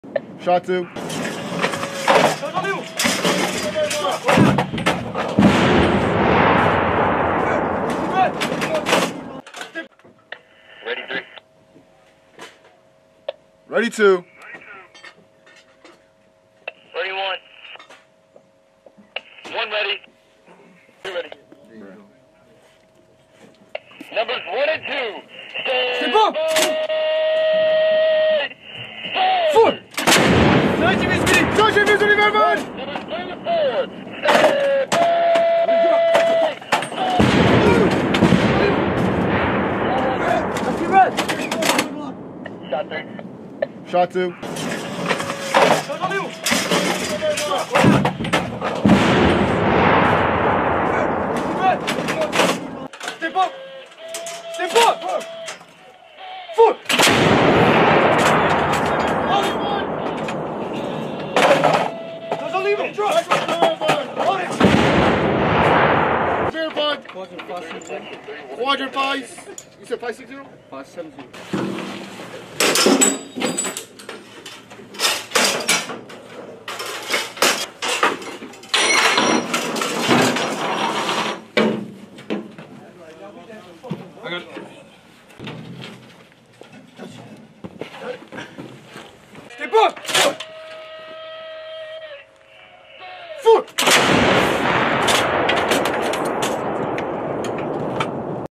Artillery Thunder: Bravo Battery Fires M198 Howitzers in Support of MCCRE
1/10 Provides fire support
CAMP LEJEUNE, NC, UNITED STATES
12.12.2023

The MCCRE involves a range of scenarios and challenges that test the unit's ability to plan, coordinate, and execute complex missions, including offensive and defensive operations, logistics, and communications. (U.S. Marine Corps video by Lance Cpl. Damian Oso)

Film Credits: Video by Lance Cpl. Damian Oso
2nd Marine Division